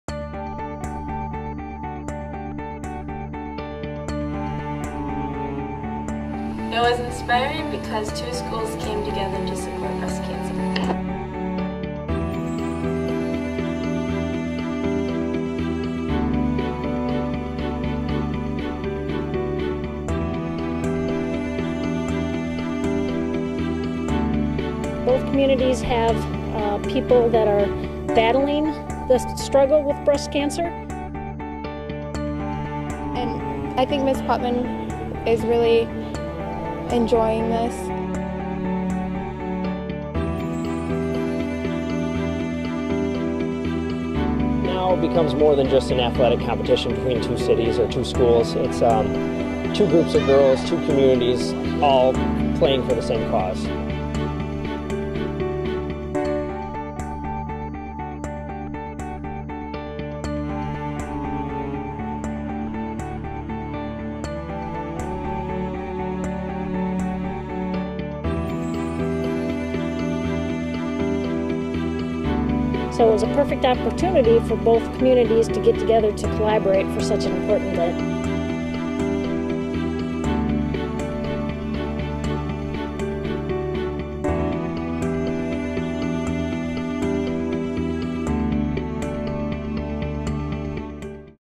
EJ vs Charlevoix - Cancer Fundraiser
The middle school basketball teams from East Jordan and Charlevoix joined forces for a common cause.  The 7th and 8th graders played each other to raise money for a Breast Cancer cure.  What great kids!